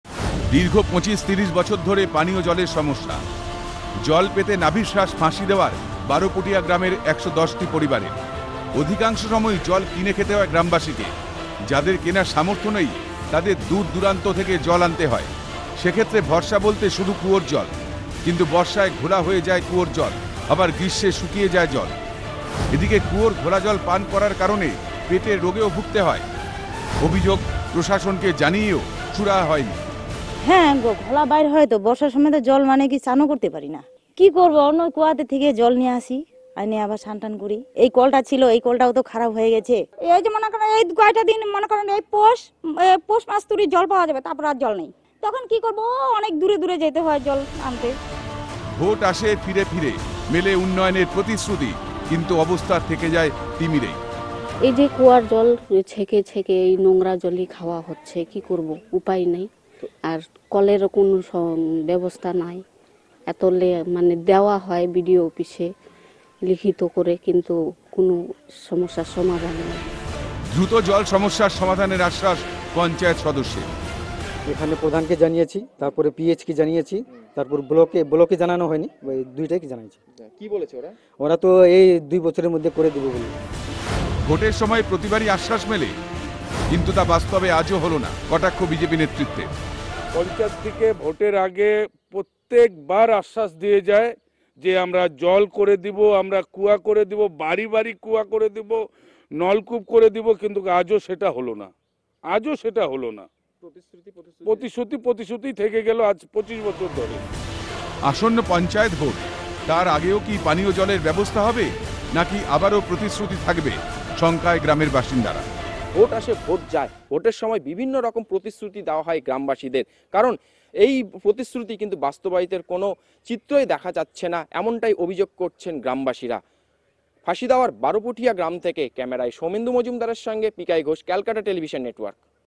পানের অযোগ্য জল | CN
সামান্য পানীয় জলটুকুও মিলছে না। ২০২২ এসেও পানীয় জল থেকে বঞ্চিত সাধারণ মানুষ। এবারে জল যন্ত্রণার আরও এক ছবি উঠে এল ফাঁসিদেওয়া ব্লকের ঘোষপুকুর অঞ্চল থেকে। CN Calcutta News